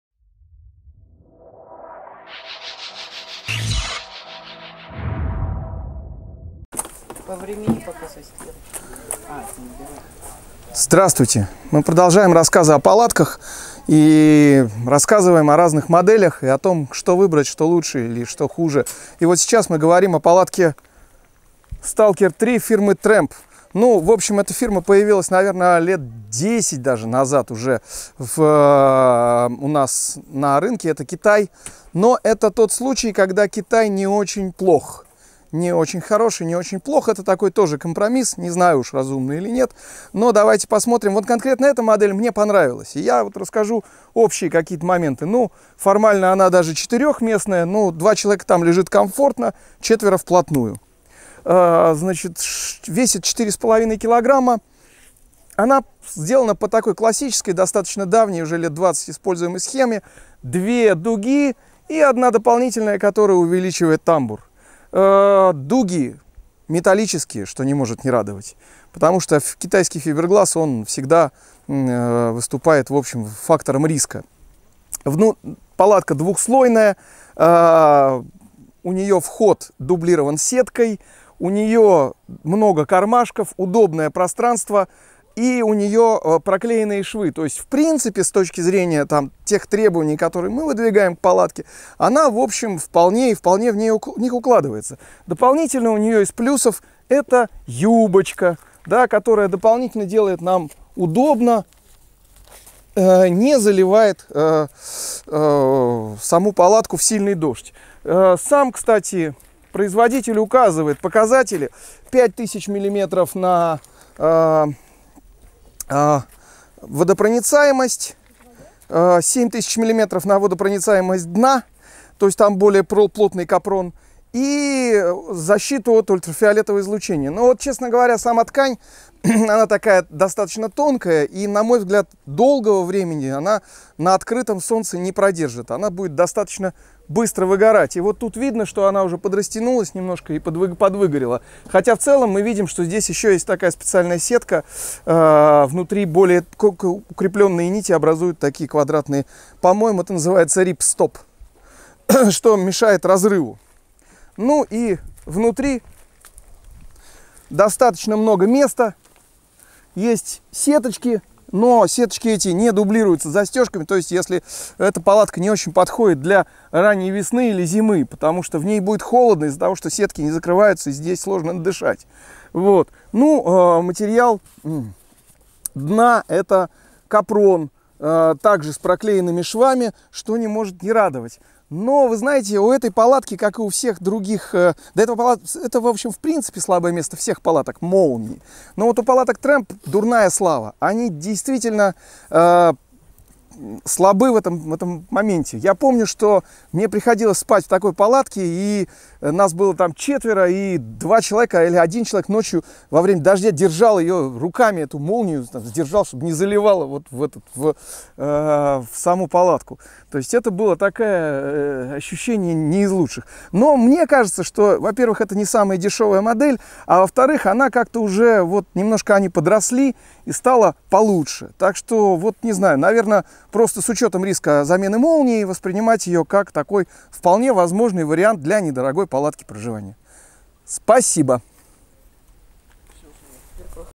Палатка Tramp Stalker 3: терпимый китай
Рассказ о палатке Tramp Stalker 3, которую мы определяем как "терпимый китай"
Палатка от фирмы Трэмп, для своих денег - приемлемого качества.
Палатка обладает для кемпинговой неплохими свойствами. Вполне подойдет и для походов, если не боитесь ее веса.
Модель не новая.
Трамп Сталкер 2 и 3 на маркете: ссылка удалена
Самая интересная закрытая тусовка по снаряжению и походам (вместе с системой поддержки нашего канала)